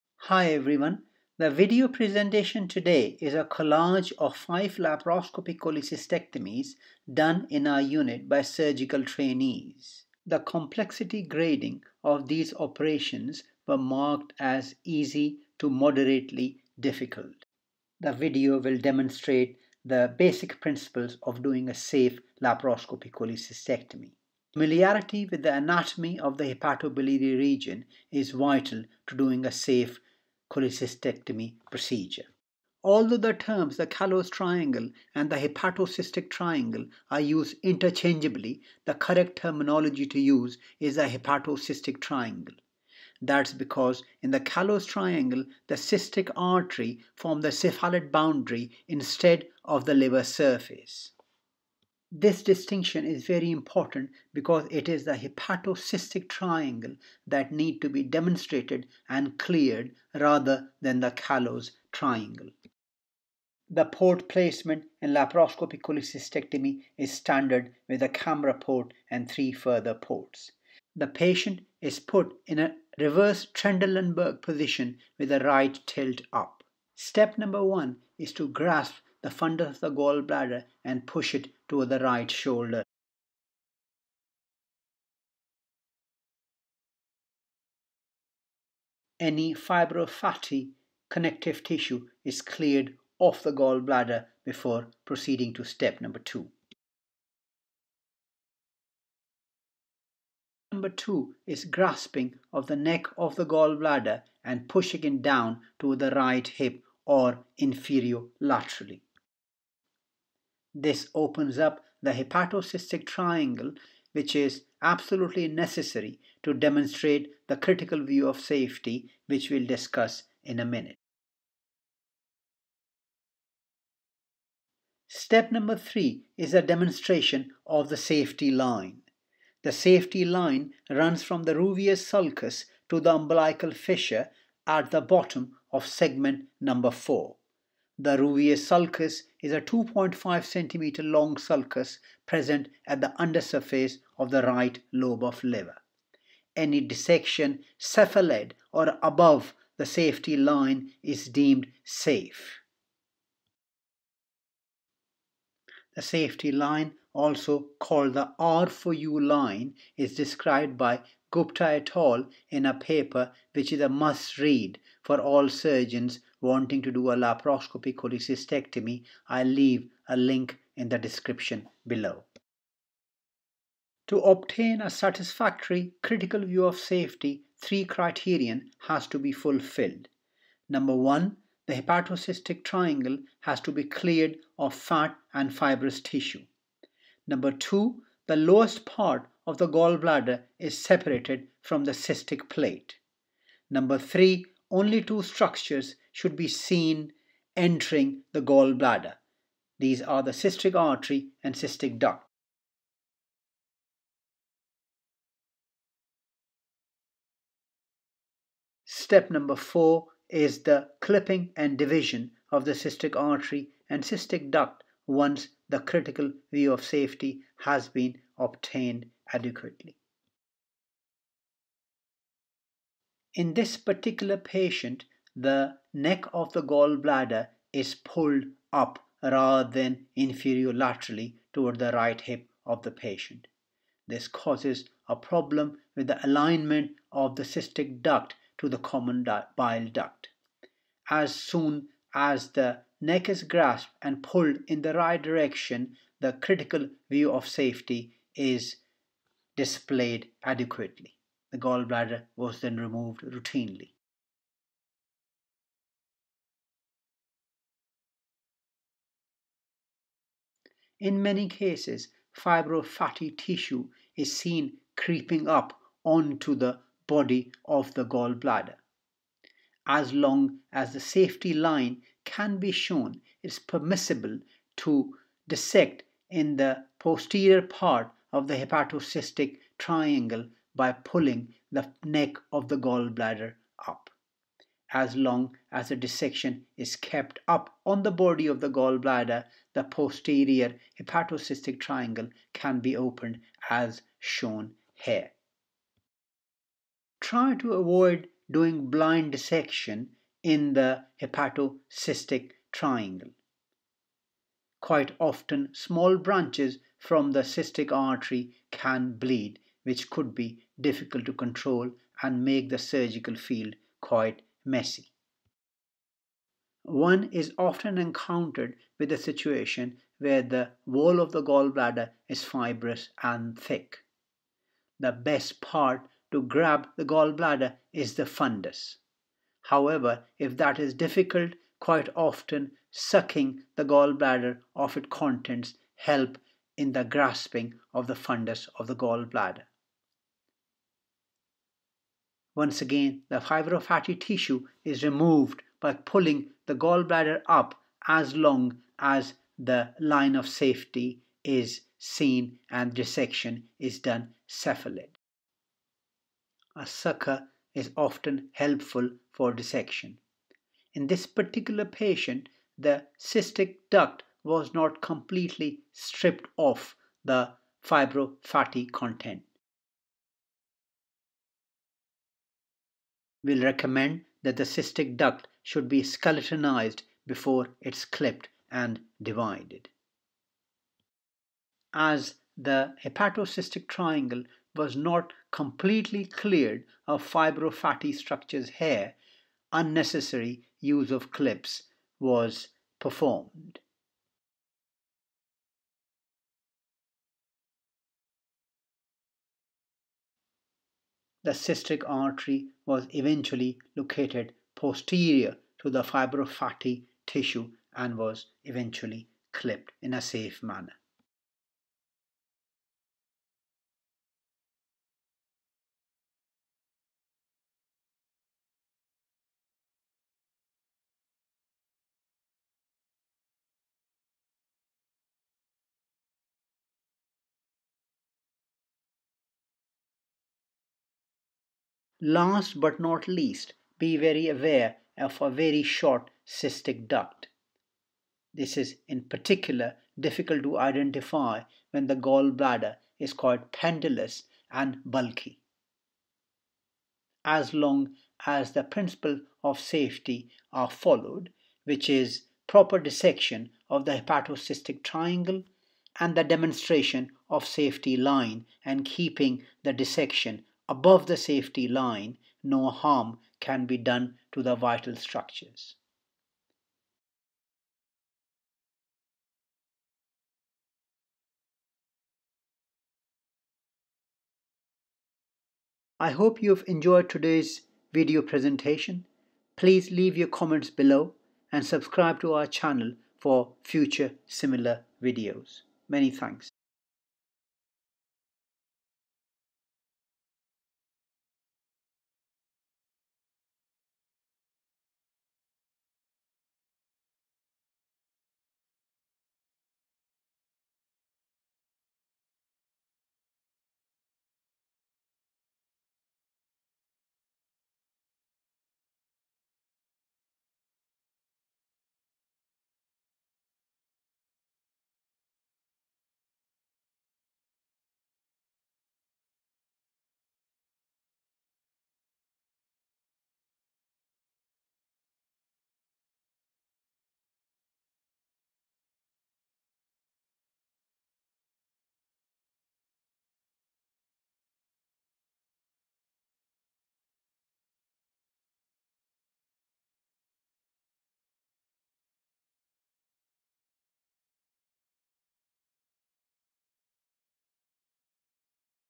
How to do a laparoscopic cholecystectomy- A step by step guide. Mr Suhail Anwar.
Lap cholecystectomy is a very commonly performed procedure. Nonetheless the incidence of CBD injury is higher in lap as compared to open cholecystectomies.
In this video we demonstrate a safe , step by step technique, of doing a lap cholecystectomy. Important land marks are defined and clearly displayed for the young surgeon to identify ad demonstrate, for a throughly efficient and safe procedure.

please check:
Safe laparoscopic cholecystectomy: Adoption of universal culture of safety in cholecystectomy by Vishal Gupta and Gaurav Jain